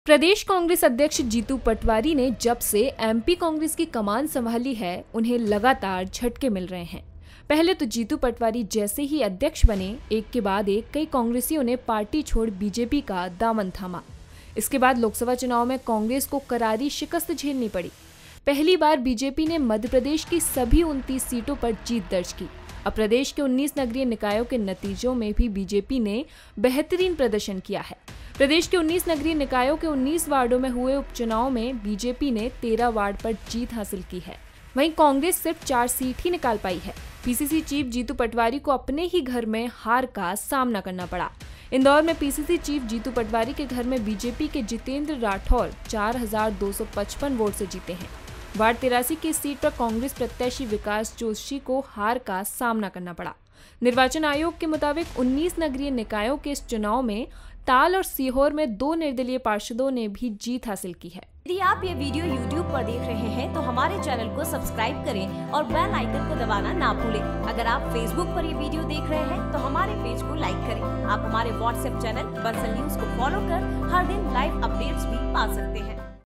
लोकसभा के बाद इस चुनाव में भी हुई कांग्रेस की करारी हार, अपने ही घर में जीत नहीं दिला पाए PCC चीफ!
प्रदेश कांग्रेस अध्यक्ष जीतू पटवारी ने जब से एमपी कांग्रेस की कमान संभाली है, उन्हें लगातार झटके मिल रहे हैं.. पहले तो जीतू पटवारी जैसे ही अध्यक्ष बने, एक के बाद एक कई कांग्रसियों ने पार्टी छोड़ बीजेपी का दामन थामा... इसके बाद लोकसभा चुनाव में कांग्रेस को करारी शिकस्त झेलनी पड़ी... पहली बार बीजेपी ने मध्यप्रदेश की सभी 29 सीटों पर जीत दर्ज की... अब प्रदेश के 19 नगरीय निकायों के नतीजों में भी बीजेपी ने बेहतरीन प्रदर्शन किया है... प्रदेश के 19 नगरीय निकायों के 19 वार्डों में हुए उपचुनाव में बीजेपी ने 13 वार्ड पर जीत हासिल की है... वहीं कांग्रेस के सिर्फ 4 सीट ही निकाल पाई है... पीसीसी चीफ जीतू पटवारी को अपने ही घर में हार का सामना करना पड़ा है... इंदौर में पीसीसी चीफ जीतू पटवारी के घर में बीजेपी के जितेंद्र राठौर 4255 वोट से जीते हैं.. वार्ड-83 की इस सीट पर कांग्रेस प्रत्याशी विकास जोशी को हार का सामना करना पड़ा...निर्वाचन आयोग के मुताबिक 19 नगरीय निकायों के इस चुनाव में ताल और सीहोर में 2 निर्दलीय पार्षदों ने भी जीत हासिल की है..